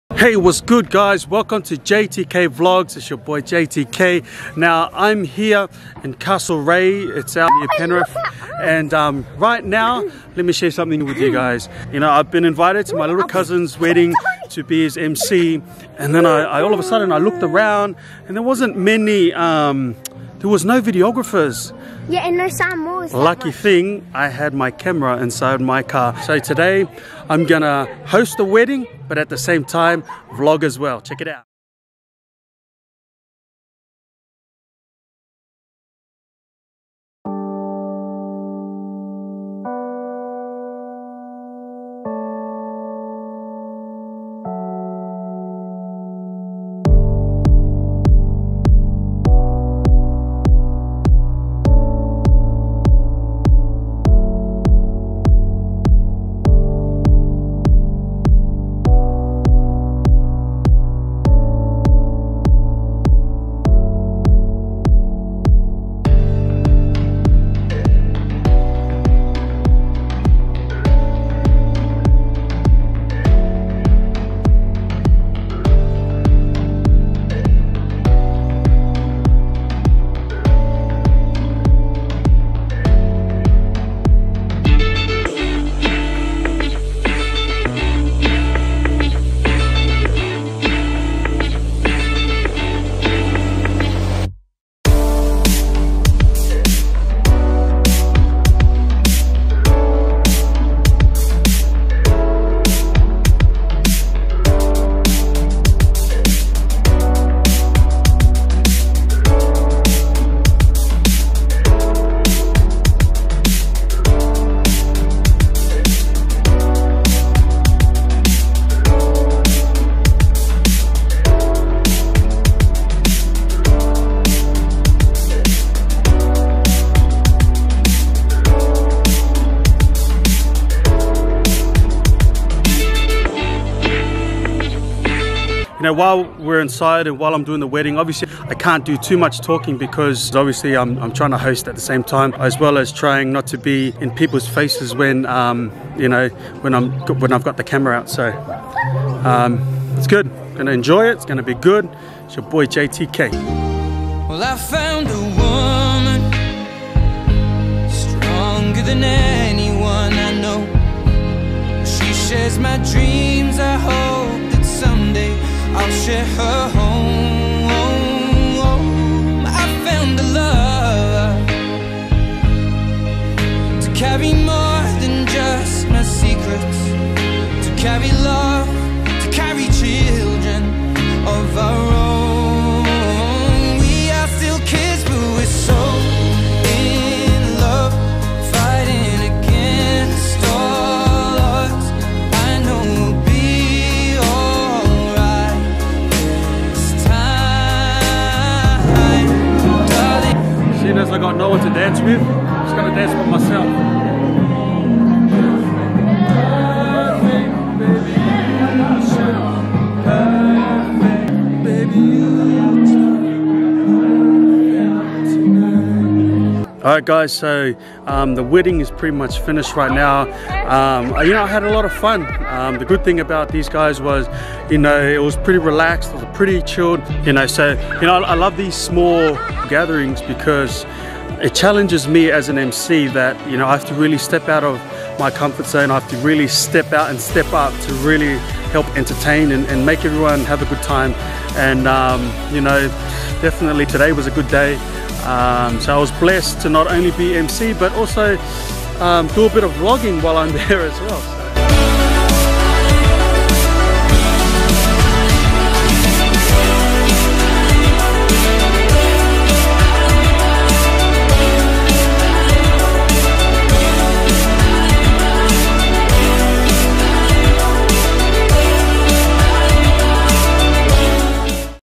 WEDDING VLOG - Hosting | Filming | Vlogging at THE SAME TIME!!!
Welcome to JTK VLOGS!

What an absolute honour it was to see my little cousin Leo marry his sweet heart Kay on the weekend 8/12/2018!!!

I was given the honour to host his wedding reception and during the ceremony, I noticed there was no videographer - Luckily I had my camera and gimbal in the car. I wish I had my proper video gear for weddings but I definitely made the most with what I had!

Sure enough I got STRAIGHT into videographer mode but had to also stay mentally sharp and focused for my MC role!
It turned out to be a night full of fun, laughter and high spirits!

Take care & God Bless

Congratulations Leo & Kay Blears on your marriage! We wish you both a life time of success & God's blessings upon you both!